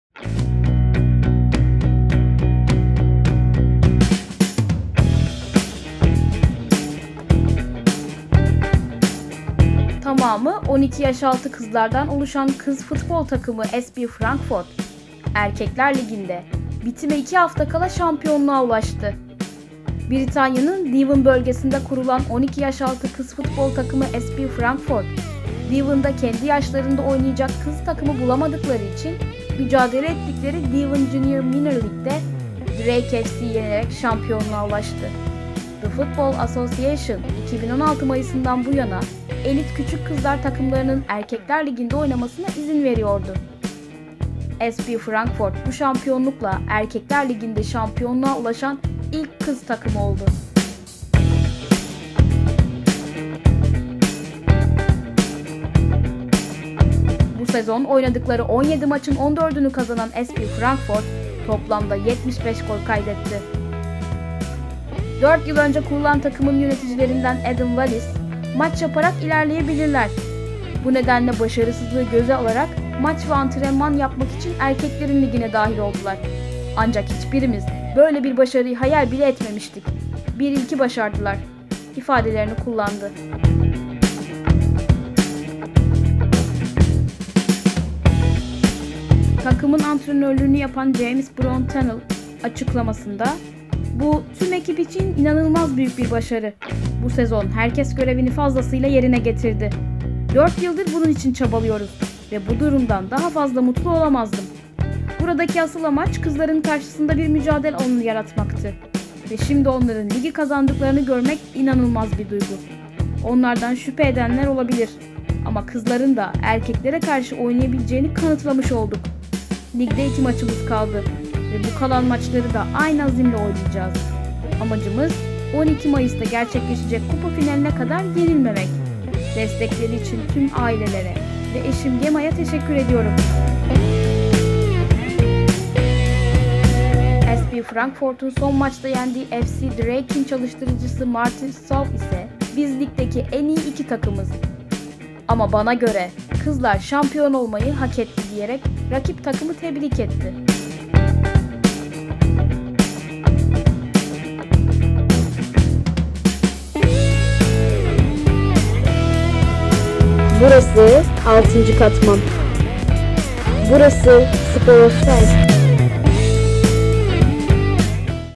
Liginde Şampiyon Oldu! / 6. Bölüm
Tamamı 12 yaş altı kızlardan oluşan kız futbol takımı SB Frankfort, erkekler liginde bitime 2 hafta kala şampiyonluğa ulaştı.

Britanya’nın Devon bölgesinde kurulan, 12 yaş altı kız futbol takımı SB Frankfort, Devon’da kendi yaşlarında oynayacak kız takımı bulamadıkları için mücadele ettikleri Devon Junior Minor League’de Drake FC’yi yenerek şampiyonluğa ulaştı.

The Football Assocatiaon (FA), 2016 Mayıs'ından bu yana elit küçük kızlar takımlarının, erkekler liginde oynamasına izin veriyordu. SB Frankfort, bu şampiyonlukla erkekler liginde şampiyonluğa ulaşan ilk kız takımı oldu. Bu sezon oynadıkları 17 maçın 14'ünü kazanan SB Frankfort, toplamda 75 gol kaydetti.